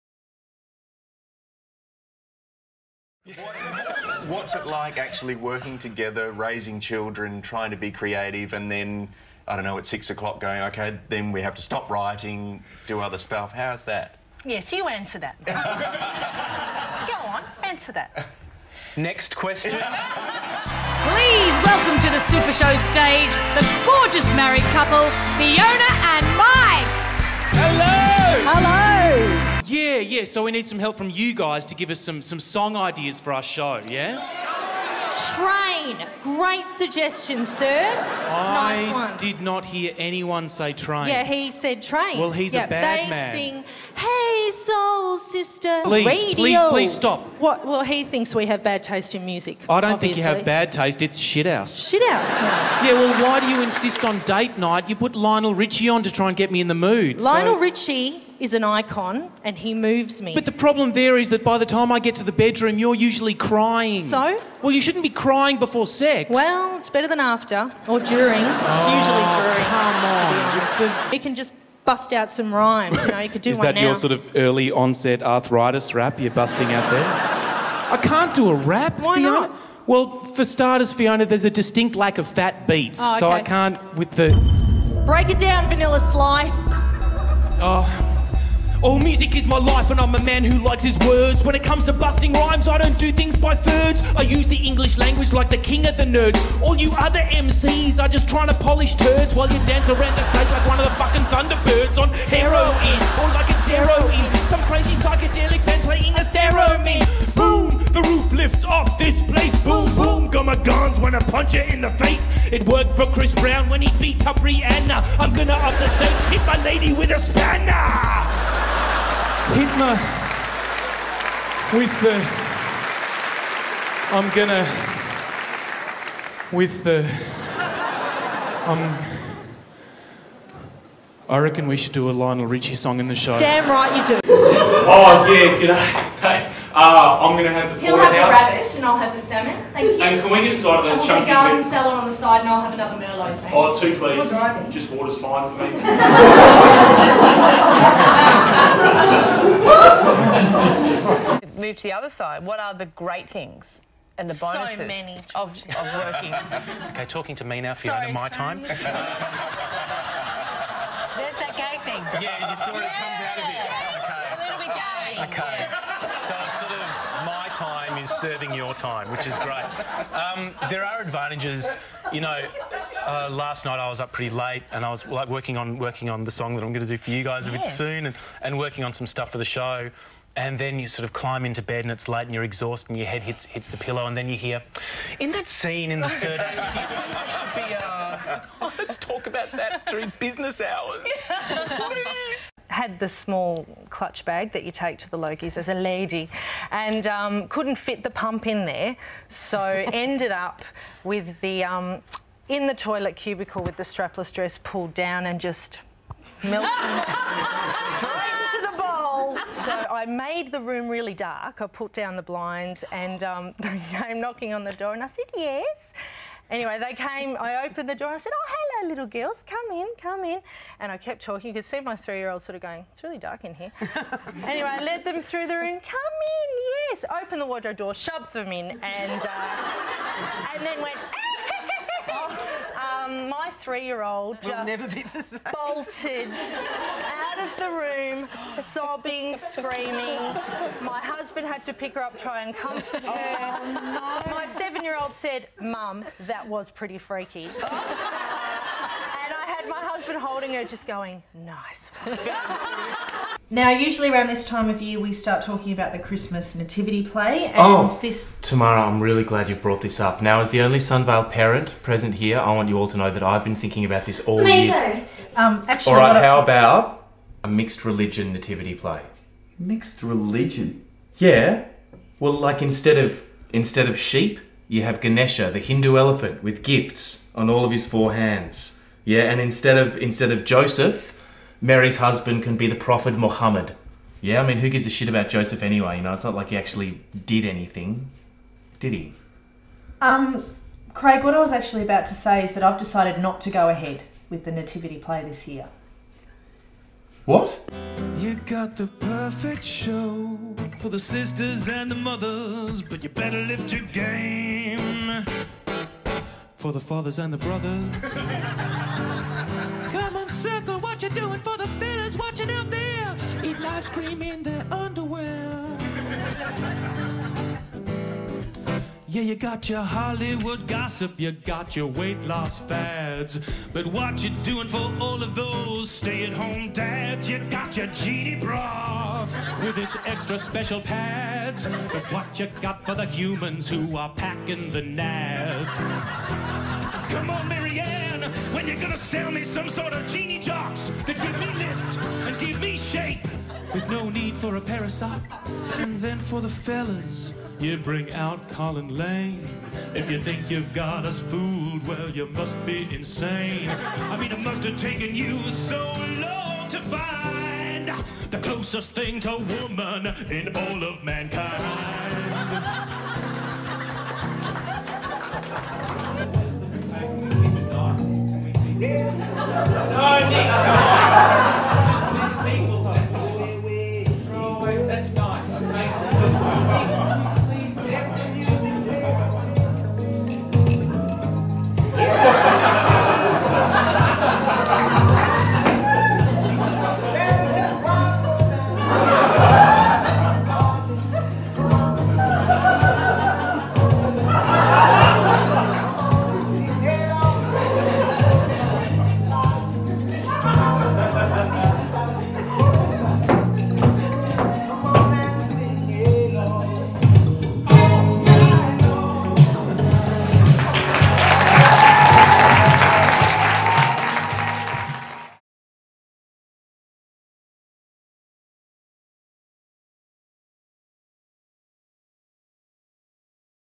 Fiona Harris & Mike McLeish 2013
Selection of highlights from performers and writers Fiona Harris & Mike McLeish